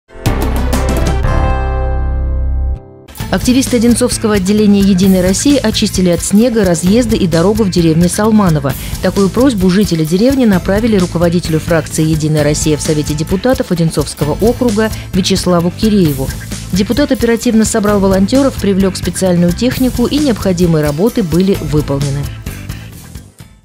Партийцы очистили от снега дорогу и разъезды в деревне Солманово
Активисты одинцовского отделения «Единой России» очистили от снега разъезды и дорогу в деревне Солманово. Такую просьбу жители деревни направили руководителю фракции «Единая Россия» в Совете депутатов Одинцовского округа Вячеславу Кирееву.